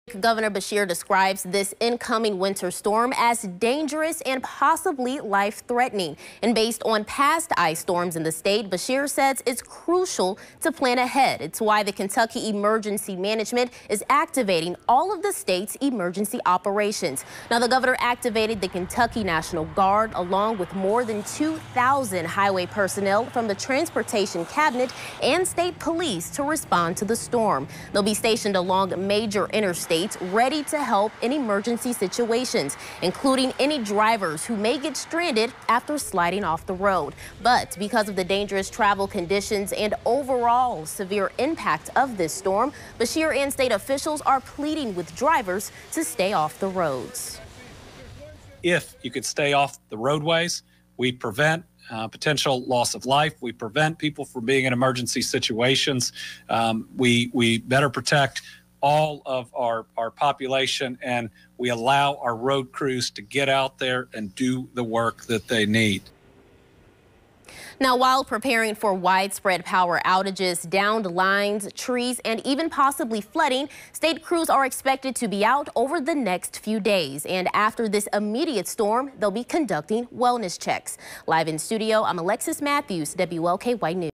State of emergency issued in Kentucky ahead of another round of winter weather
Gov. Andy Beshear has declared a State of Emergency ahead of a "dangerous" icy winter storm coming through in the next couple of days.